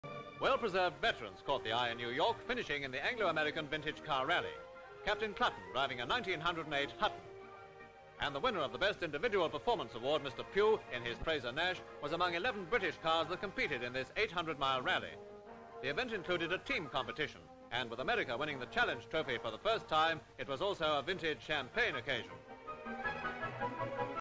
USA: NEW YORK: BRITISH VETERAN CARS: (1957)
NEW YORK: BRITISH VETERAN CARS:

Full Description:

America. Vintage Car Rally in New York...11 British Cars compete

CARS. English and American cars in Vintage car Rally in New York shot of 1908 Hutton and 1928 Frazer-Nash.

NEW YORK. Anglo-American Village Car Rally in..A 1908 Hutton driven by Captain Clutton..and Mr Pugh in his 1927 Frazer-Nash wins the award for the best individual performance.

Vehicles; United States of America
Background: NEW YORK: BRITISH VETERAN CARS: